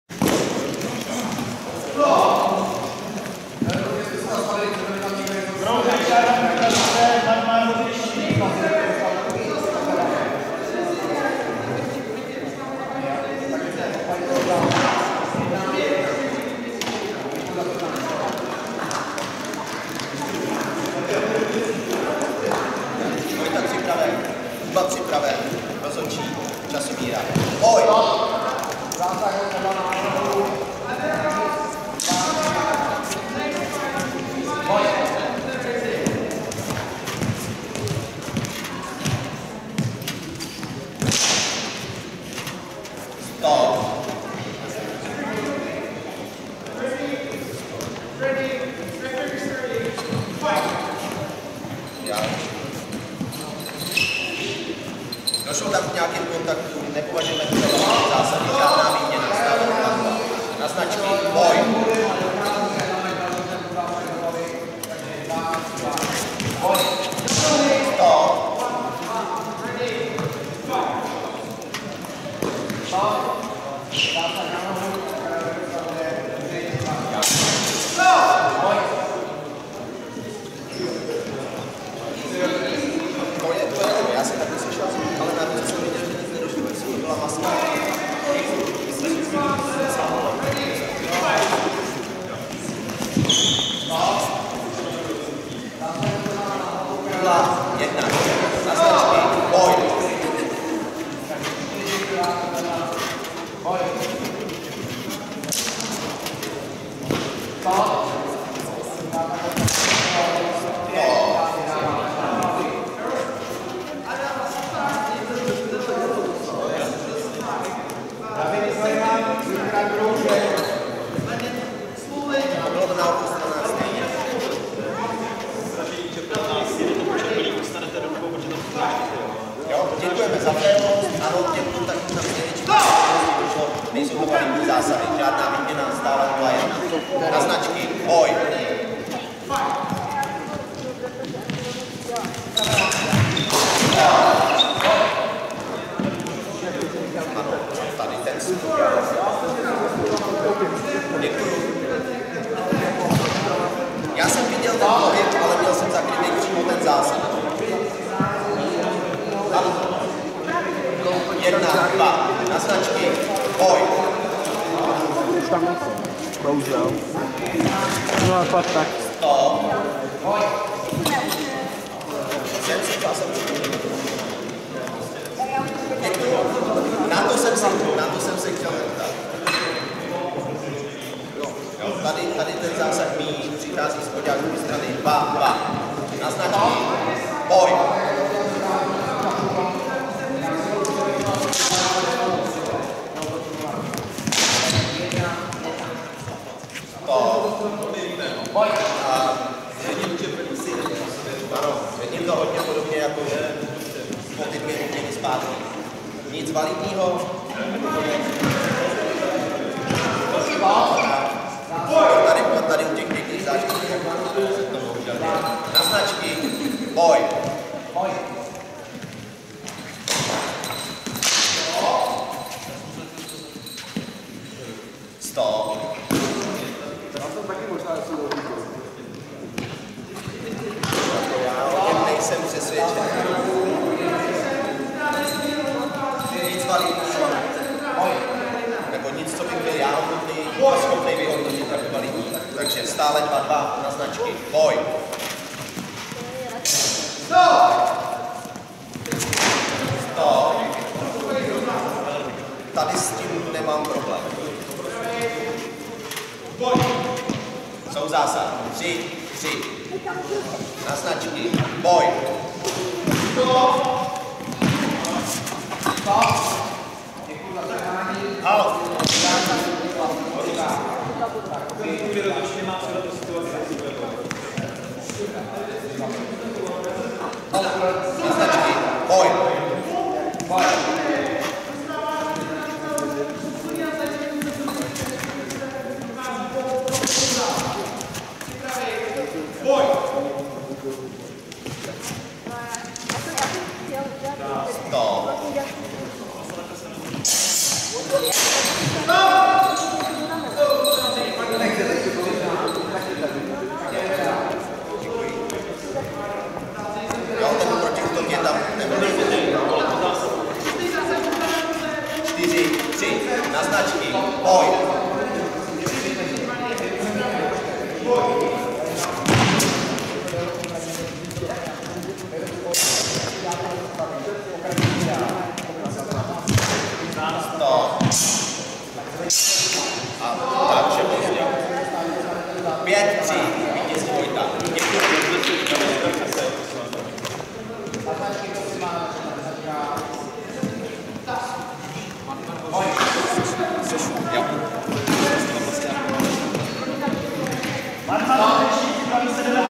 Podzimové mečoklání II 2022 - plasty skupina č.1 - Vojtěch Kroužek vs. Kuba Šarhan
Dlouhý meč

Turnaj Podzimové mečoklání II proběhl v Praze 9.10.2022 ve sportovní hale Radotín. Účastnilo se celkem 68 závodníků a závodnic z česka a jeden závodník ze slovenska. Již tradičně jsme krom disciplín na železo pro dospělé: Dlouhý meč open a Dlouhý meč ženy,  otevřeli i dvě disciplíny pro děti a mládež a to Plasty a Bacátka.